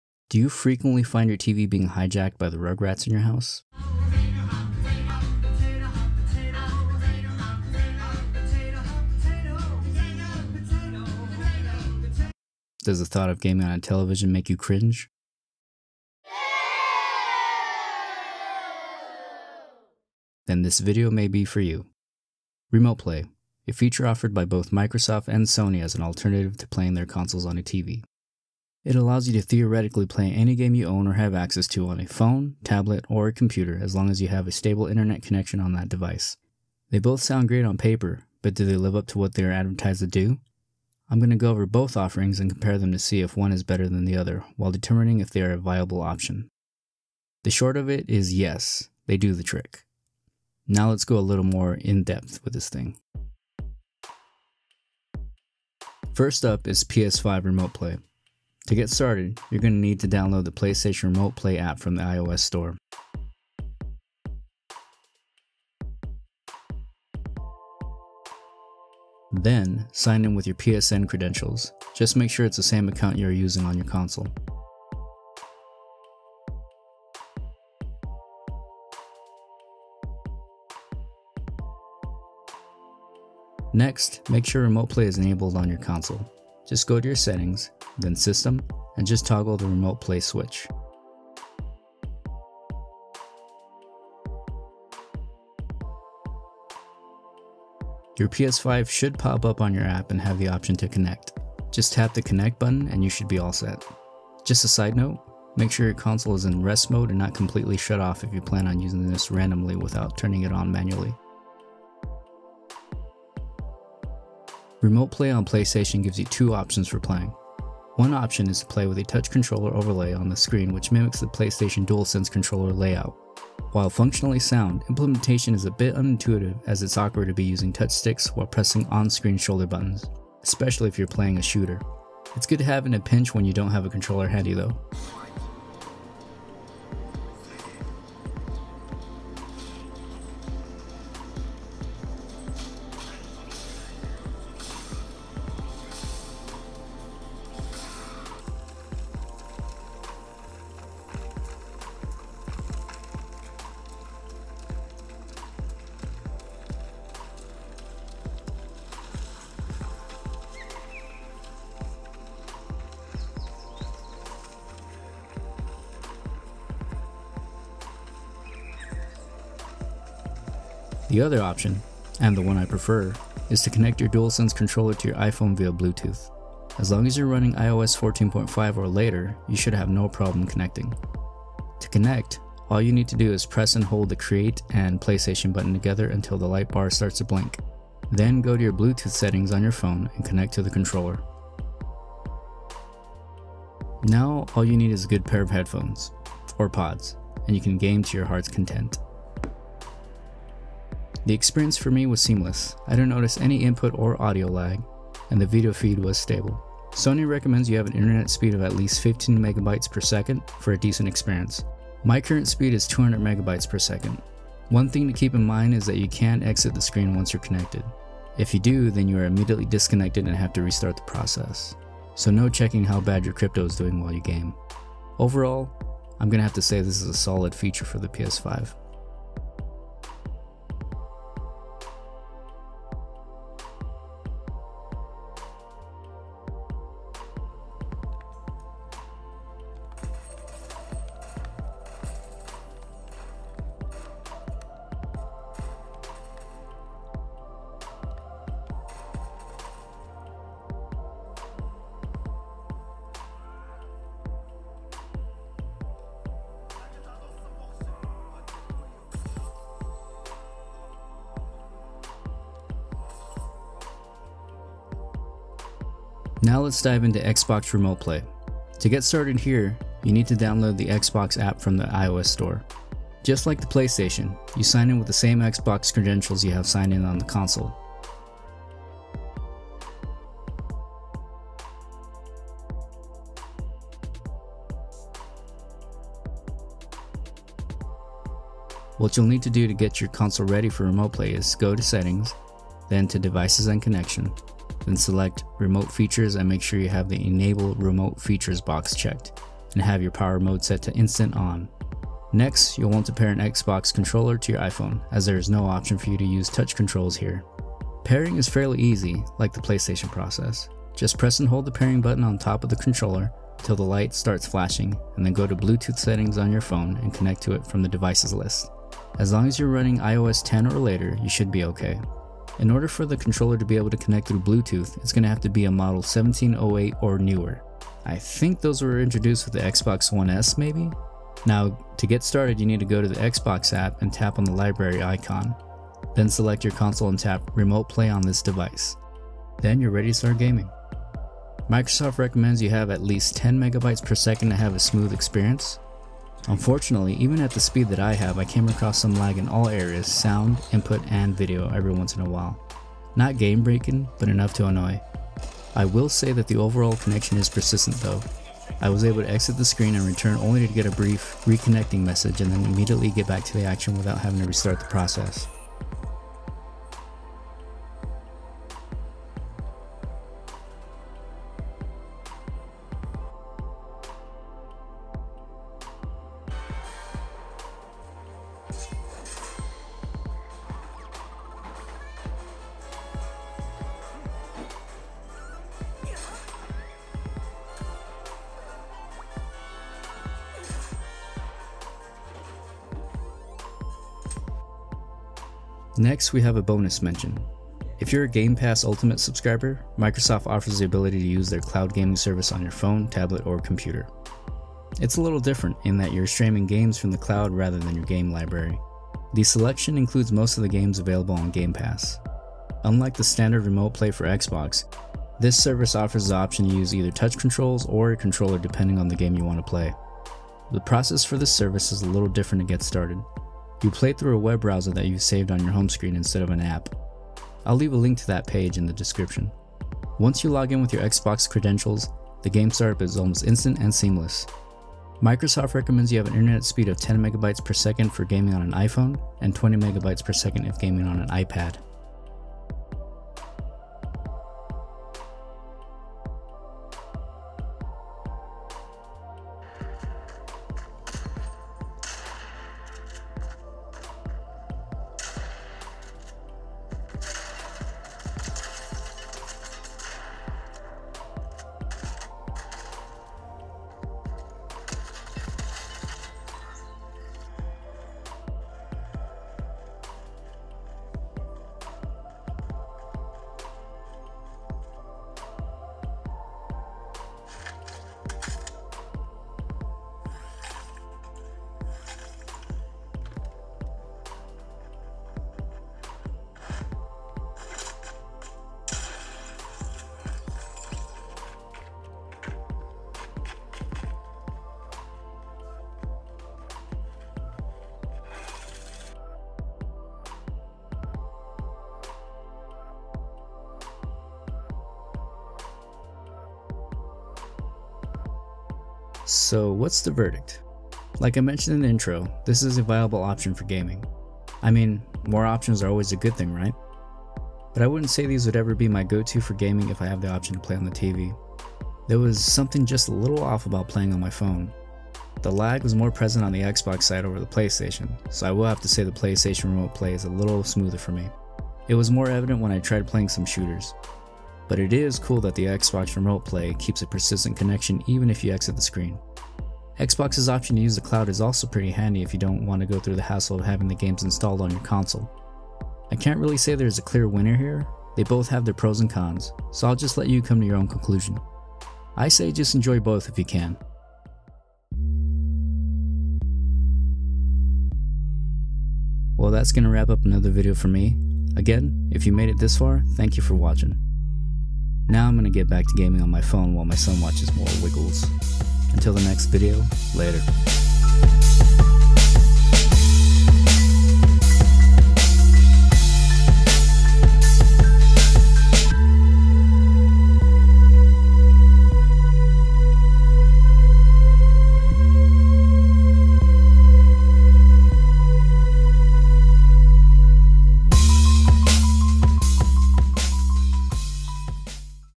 Remote Play: Is it Any Good? | PS5 Vs. Xbox Series X
In this video I take a look at the remote play options offered by both Microsoft and Sony and discuss their pros and cons. Does your television actually have competition? We shall see.

00:00 - Intro
00:55 - PS5 Remote Play
02:51 - PS5 Remote Play w/Controller
04:25 - Xbox Remote Play
06:35 - Xbox Cloud Gaming
08:19 - Xbox Cloud Gaming w/Controller
09:15 - Outro